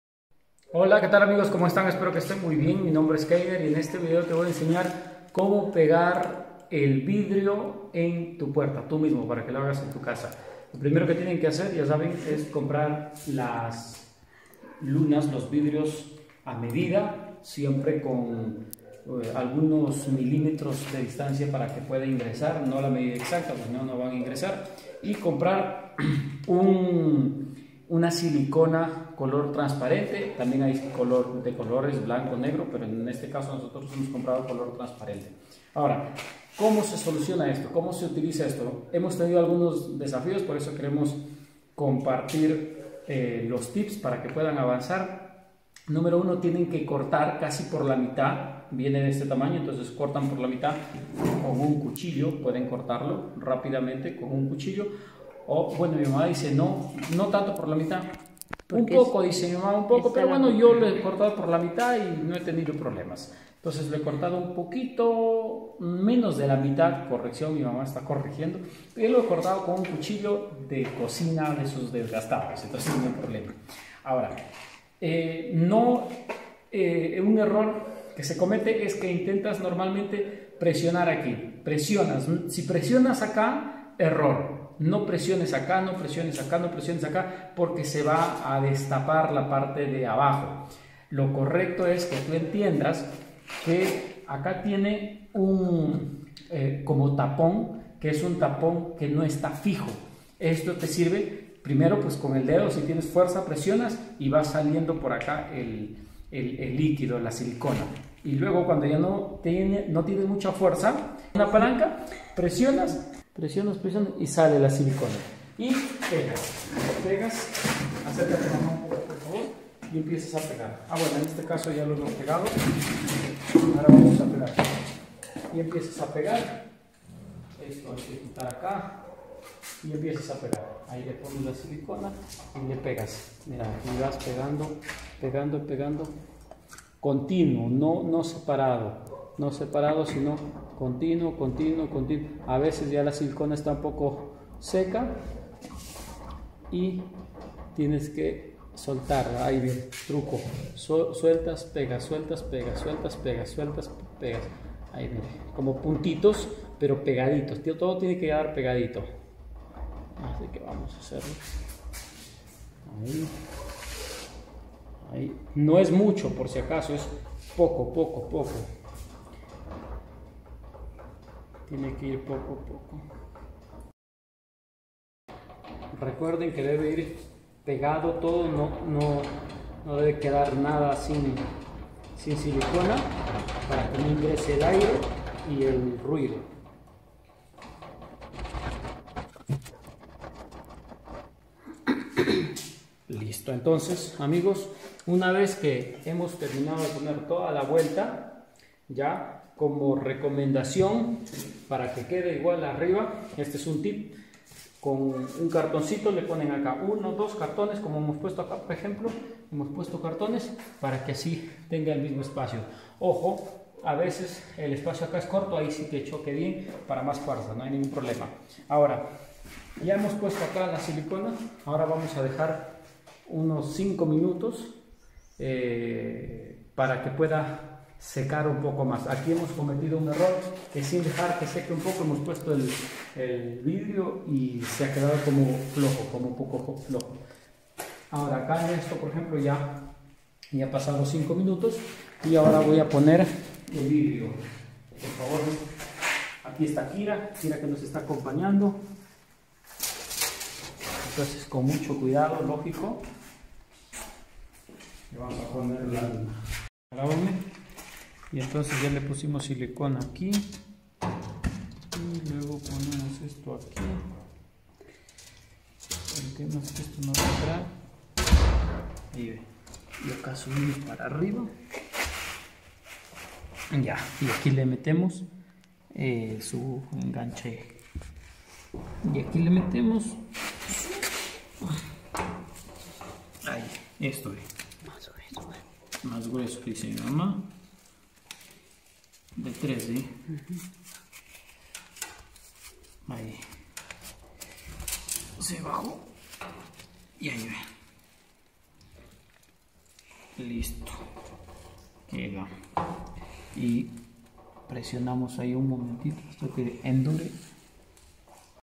Como pegue el Vidrio con silicona transparente en Puerta de Fierro - Fácil y rápido
Sígueme, comparte y comenta…